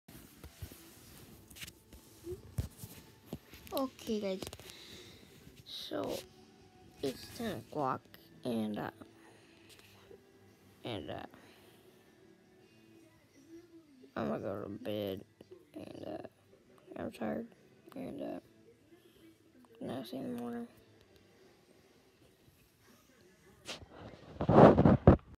heure du coucher
It was 10:00. Kinda hard to sleep, just had the creeps, but I managed